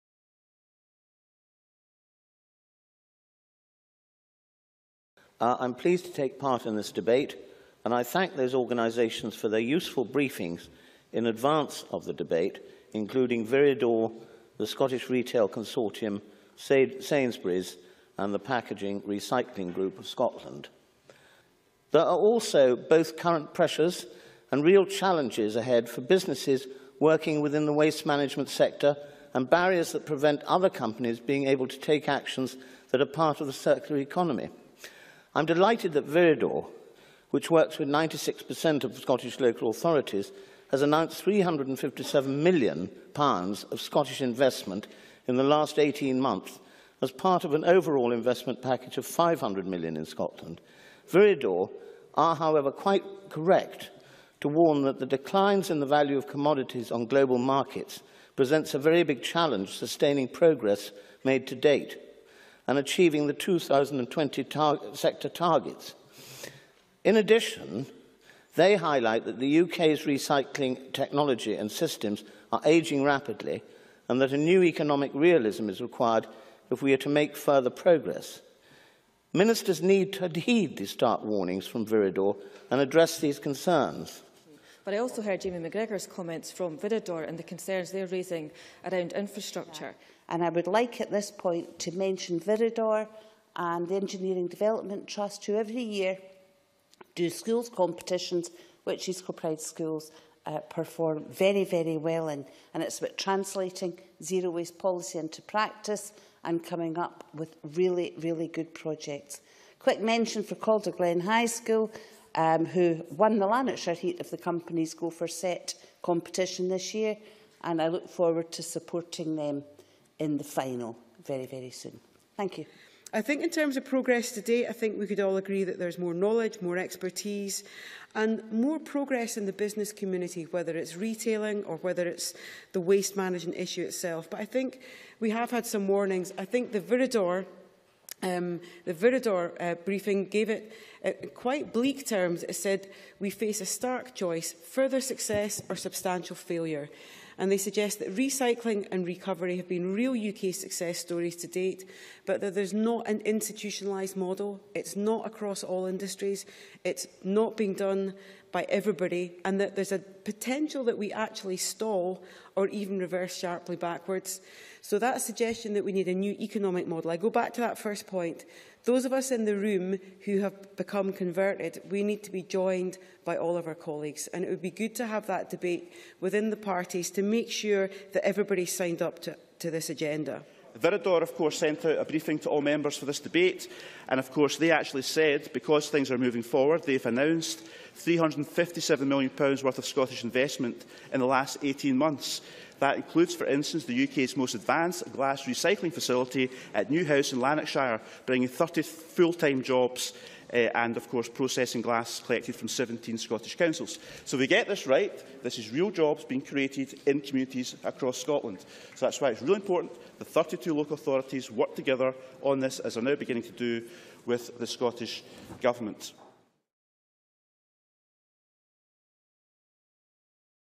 Scottish Parliament Circular Economy (Waste) Debate May 2015
The Scottish Parliament debated the circular economy (waste) on 14th May 2015.  Find out what members had to say on Viridor's £500m Scottish investment programme and on the challenges and opportunities for the sector.